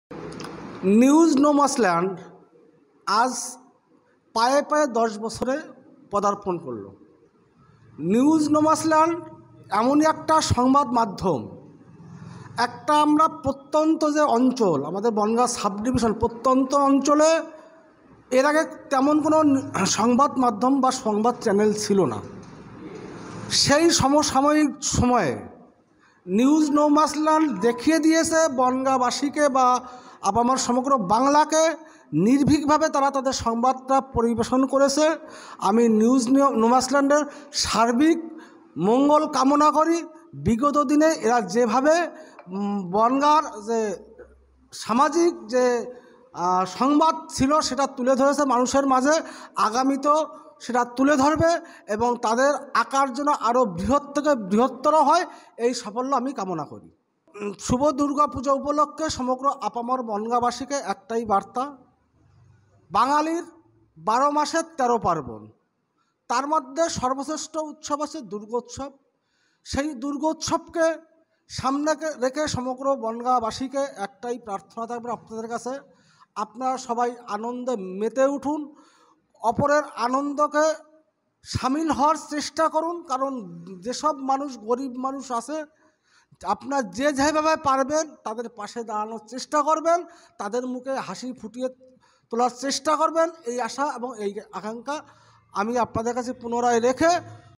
আপনাদের ভালোবাসায় নিউজ নো ম্যানস্ ল্যান্ড এখন ১০বছরে৷ শুভেচ্ছায় বিধায়ক, অশোক কীর্তনীয়া।
পায়ে পায়ে ১০ এ, আপনাদের ভালোবাসায় নিউজ নো ম্যানস্ ল্যান্ড এখন ১০বছরে৷এভাবেই সাথে থাকুন , পাশে থাকুন ৷ শুভেচ্ছায় বিধায়ক, অশোক কীর্তনীয়া।